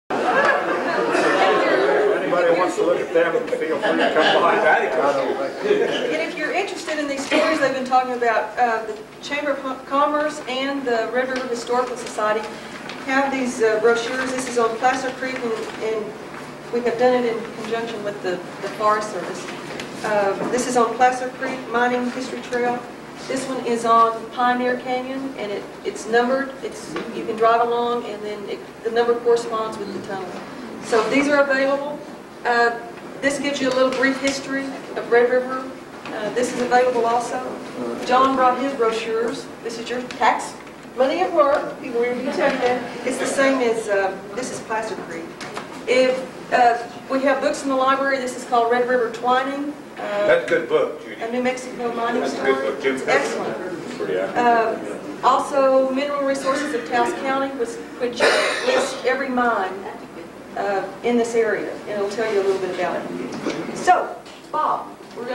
Jb12 27 52 28 57 judy brochures
Another panel arranged by Judy Brunson during her service as the Director of the Red River Historical Society in 1994.  Listen to John Young (Carson NF Archeologist), Bob Tiffany (ex-MolyCorp manager), John Brandenburg, John Miller, Johnnie Mutz, Harold Young & Bob Prunty tell their stories of the mining days of Red River.
Paul Smith